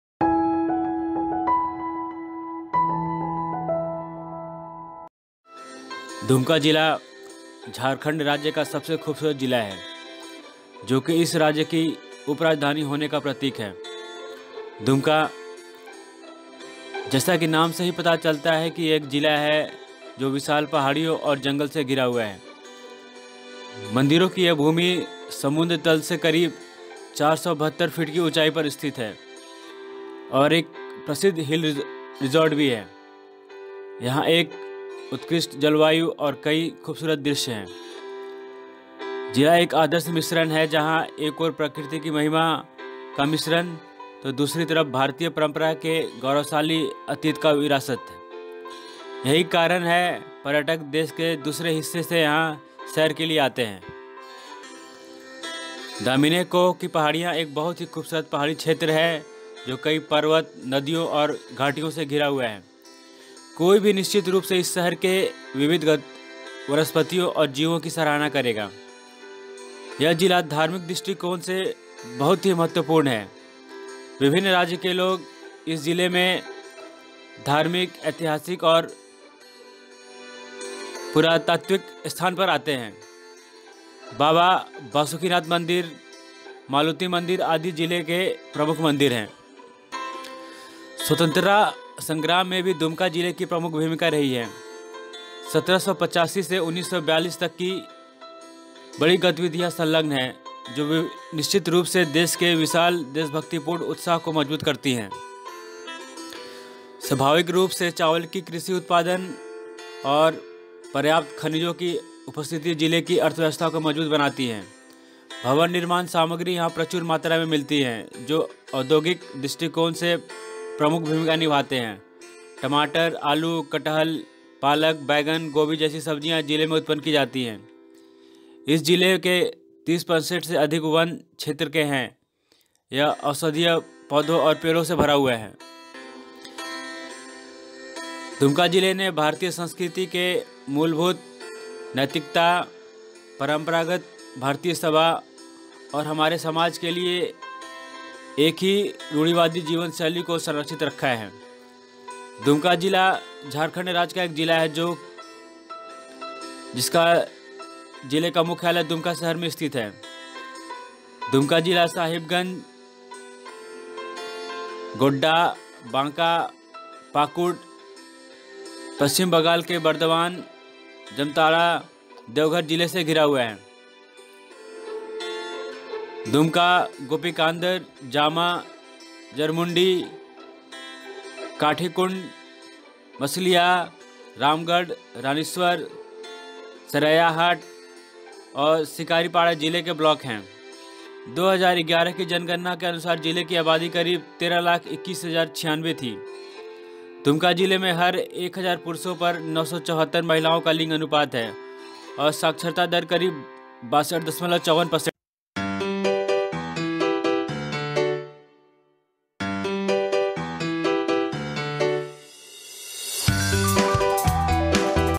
दुमका झारखंड | Dumka District Jharkhand | Dumka City | My City My Life.
दुमका जिला झारखंड राज्य का सबसे खूबसूरत जिला है जो कि इस राज्क उप-राजधानी होने का प्रतीक है। दुमका – जैसा कि नाम से पता चलता है कि एक जिला है, जो विशाल पहाड़ियों और जंगल से घिरा हुआ है । मंदिरों की यह भूमि समुद्र तल से 472 फीट की ऊंचाई पर स्थित और एक प्रसिद्ध हिल रिसोर्ट भी है। यहाँ एक उत्कृष्ट जलवायु और कई खूबसूरत दृश्य हैं। जिला एक आदर्श मिश्रण है जहां एक ओर  प्रकृति की महिमा का मिश्रण तो दूसरी तरफ भारतीय परंपरा के गौरवशाली अतीत की विरासत। यही कारण पर्यटक देश के दूसरे हिस्से से यहाँ सैर के लिए आते हैं।

“दामिन-ए-कोह” की पहाड़ियों एक बहुत ही खूबसूरत पहाड़ी छेत्र हैं जो कई पर्वत, नदियों और घाटियों से घिरा हुआ है। कोई भी निश्चित रूप से इस शहर के विविध वनस्पतियों और जीवों की सराहना करेगा।
यह जिला धार्मिक दृष्टिकोण से बहुत महत्वपूर्ण है विभिन्न राज्य के लोग इस जिले के धार्मिक, ऐतिहासिक और पुरातात्विक स्थान पर आते हैं। बाबा बसुकिनाथ मंदिर, मालुति मंदिर आदि जिले के प्रमुख मंदिर हैं।

स्वतंत्रता संग्राम में भी दुमका जिले की प्रमुख भूमिका रही है। 1785 से 1942 तक, बड़ी गतिविधियां संलग्न हैं जो निश्चित रूप से देश के विशाल देशभक्तिपूर्ण उत्साह को मज़बूत करतीं हैं।

स्वाभाविक रूप से चावल कृषि उत्पादन और पर्याप्त खनिजों की उपस्थिति जिले  की अर्थव्यवस्था को मज़बूत बनती हैं। भवन निर्माण सामग्री यहां प्रचुर मात्रा में मिलतीं हैं जो औद्योगिक दृष्टिकोण से प्रमुख भूमिका निभाते हैं। टमाटर, आलू, कटहल, पालक, बैंगन,गोभी जैसी सब्जियां जिले में उत्पन की जातीं हैं। इस जिले के 30% से अधिक वन क्षेत्र है यह औषधीय पौधों और पेड़ से भरा हुआ है।

दुमका जिले ने भारतीय संस्कृति के मूलभूत नैतिकता, परंपरागत भारतीय सभा और हमारे समाज के लिए एक ही रूढ़िवादी जीवन शैली को संरक्षित रखा है।

दुमका जिला, झारखंड राज्य का एक जिला है और जिले का मुख्यालय दुमका सहर में स्तिथ है। दुमका जिला साहिबगंज, गोड्डा,बांका, पाकुड़, पश्चिम बंगाल के बर्दवान , जामताड़ा, देवघर जिले से घिरा हुआ है।

दुमका, गोपीकांदर, जामा, जरमुंडी,काठीकुंड,मसलिया,रामगढ,रानीश्वर,सरैयाहाट और शिकारीपाड़ा जिले के ब्लॉक हैं।

2011  की जनगणना  के अनुसार जिले की आबादी करीब13,21,096 थी। दुमका जिले में हर 1000 पुरुषों पर 974 महिलाओं का लिंग अनुपात है,और साक्षरता दर 62.54% है।

Note :- Images used in this video are from advance search, credit given to the person who originally captured the images. This video is uploaded for education and information purpose.